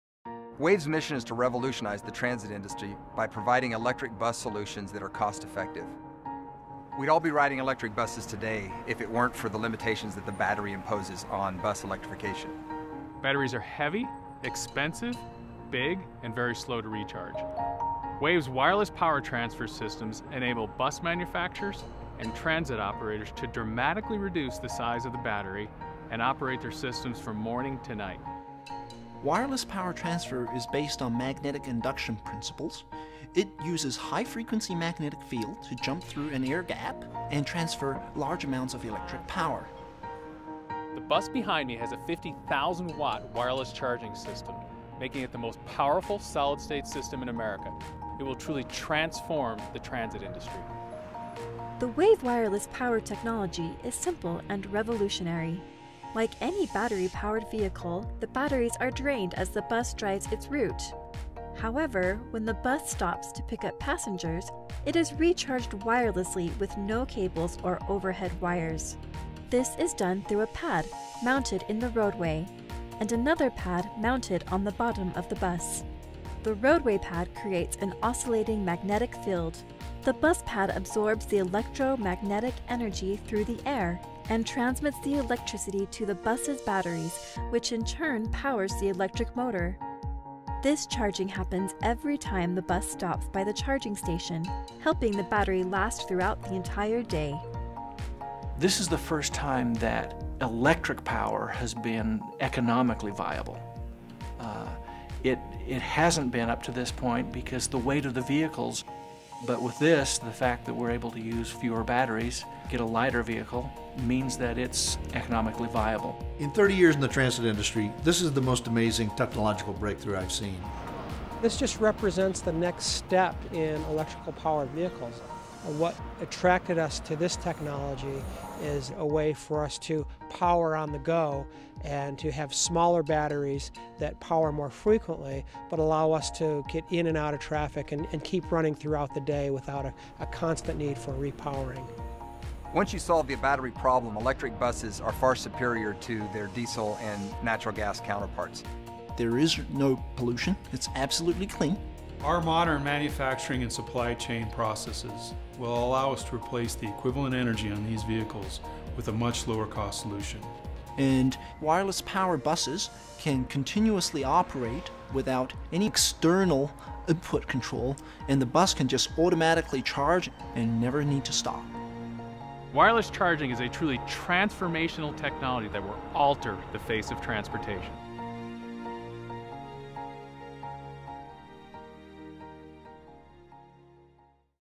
Complete Coach Works ZEPS Electric bus featuring WAVE charging techology
Its a new WAVE of technically advanced transportation that will revolutionize the industry of Public Transit. You've may have seen the Electric bus that was featured on Jay Leno's Garage, well this is the third Generation of Electric buses produced by Complete Coach Works.